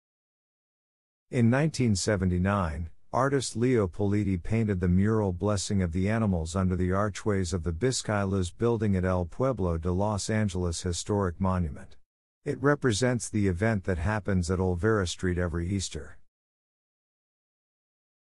Blessing of the Animals Mural
In 1979, artist Leo Politi painted the mural "Blessing of the Animals" under the archways of the Biscailuz Building at El Pueblo de Los Angeles Historic Monument. It represents the event that happens at Olvera Street every Easter..